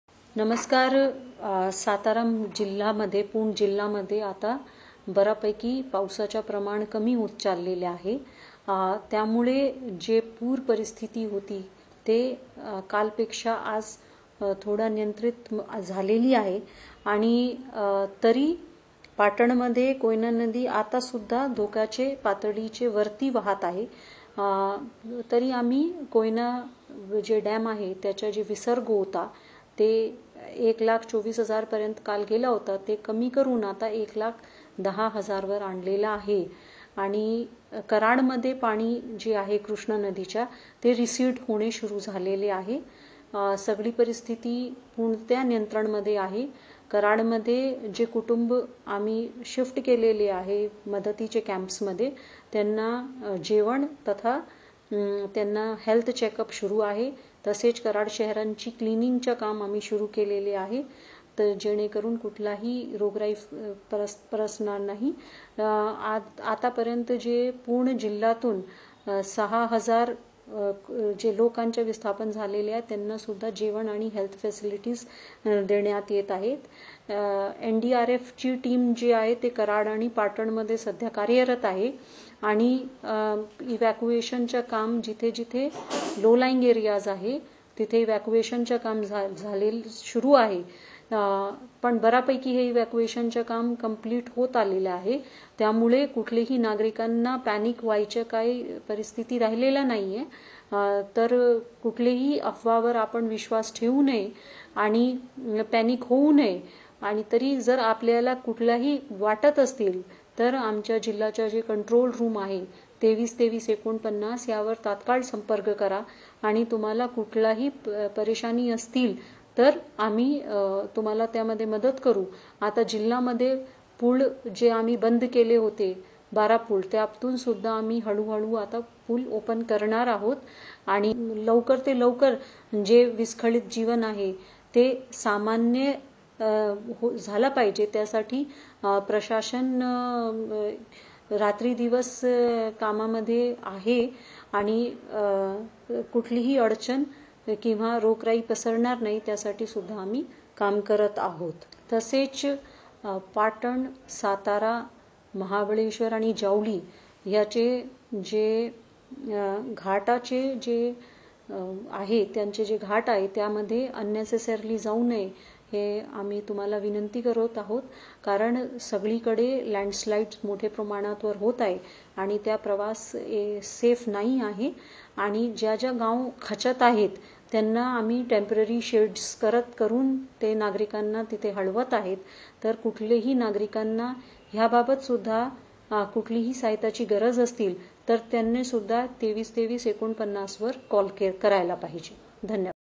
सातारा जिल्ह्यातील पूरपरिस्थिती नियंत्रणात
*सातारा जिल्ह्यातील पूरपरिस्थिती आता नियंत्रणात आली असून धरणातील पाण्याचा विसर्गही कमी केला आहे.  लोकांनी कोणत्याही अफवांवर विश्वास   ठेवू नये जिल्हाधिकारी श्वेता सिंघल यांचे जिल्ह्यातील जनतेला आवाहन*